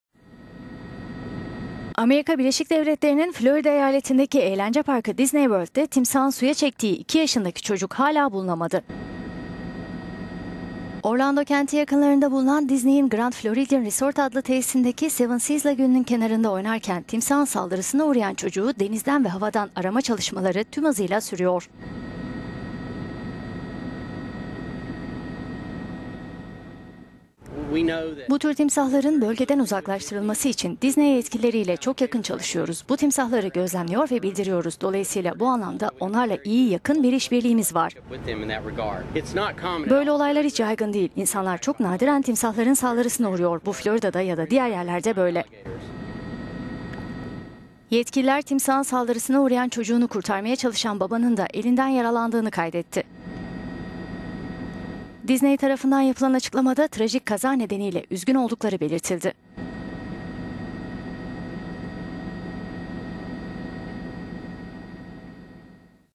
Disney World'de eğlence dehşete dönüştü
Amerikan Birleşik Devletleri'nin (ABD) Florida eyaletindeki eğlence parkı Disney World'da timsahın suya çektiği iki yaşındaki çocuk hala bulunamadı. Orlando kenti yakınlarında bulunan Disney'in Grand Floridian Resort & Spa adlı tesisindeki Seven Seas Lagünü'nün kenarında oynarken timsahın saldırısına uğrayan çocuğu arama çalışmaları denizden ve havadan tüm hızıyla devam ediyor.

Florida Balık ve Yaban Hayatı Koruma Komisyonu'ndan Nick Wiley bu tür olayların çok nadiren görüldüğünü söyledi: "…